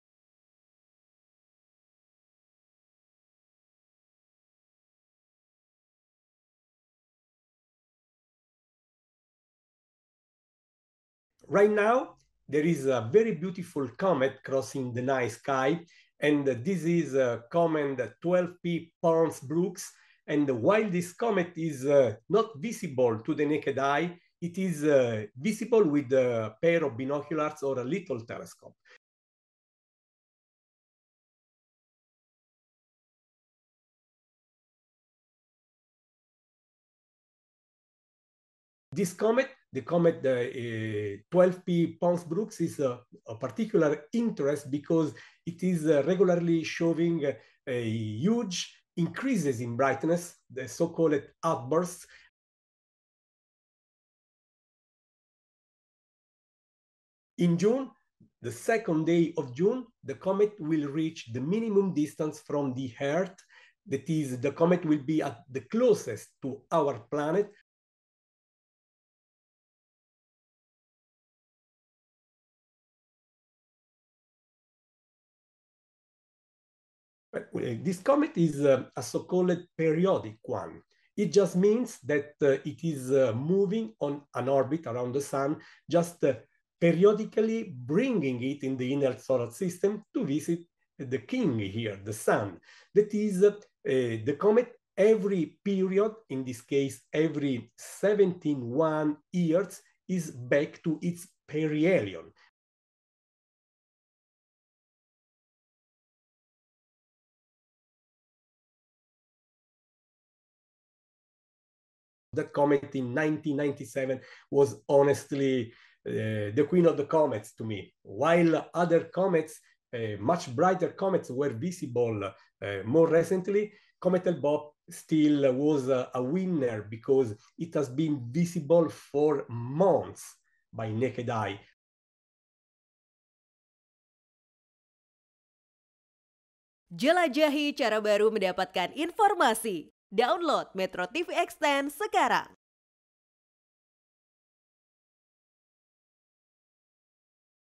Komet 12P/Pons-Brooks Mendekati Bumi
MetroTV, Salah satu komet periodik terang yang diketahui - Komet 12P/Pons-Brooks sedang menuju ke arah kita.

Komet akan mencapai titik terdekatnya dengan matahari pada 21 April dan akan paling dekat dengan Bumi pada 2 Juni.

Meskipun tidak akan terlihat dengan mata telanjang, para penggemar astronomi akan dapat melihatnya dengan teropong atau teleskop kecil.

Sumber : GIANLUCA MASI - VIRTUAL TELESCOPE PROJECT